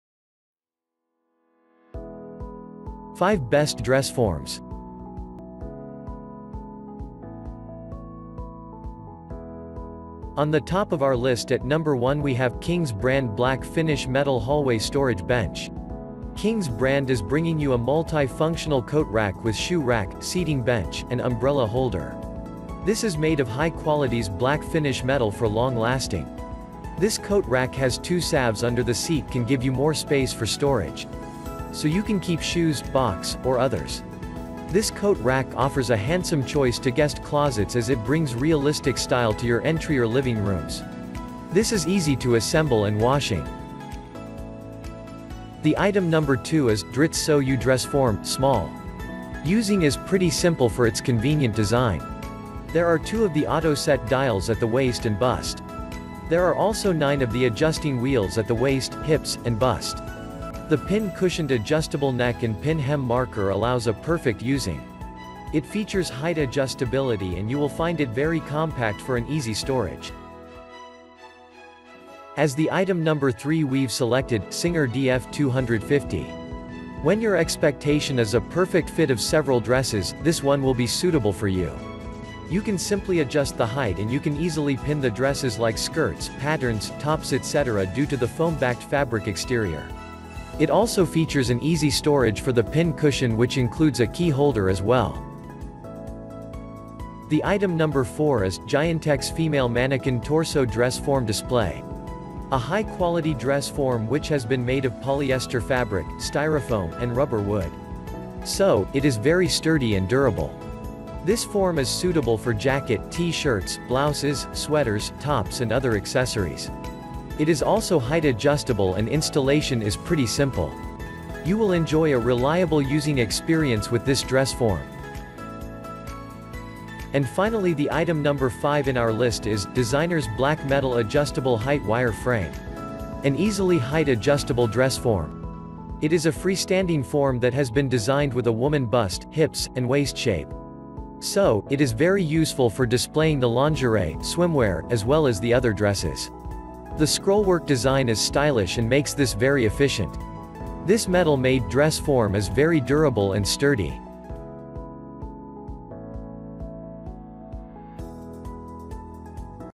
Best Dress Forms
The Included Items are,
✅5. Designers Black Metal Adjustable Height Wire Frame
✅4. Giantex Female Mannequin Torso Dress Form
✅3.SINGER DF250
✅2. Dritz Sew You Dress Form, Small
✅1.White Female Dress Form

You will get update for the top products list in our website.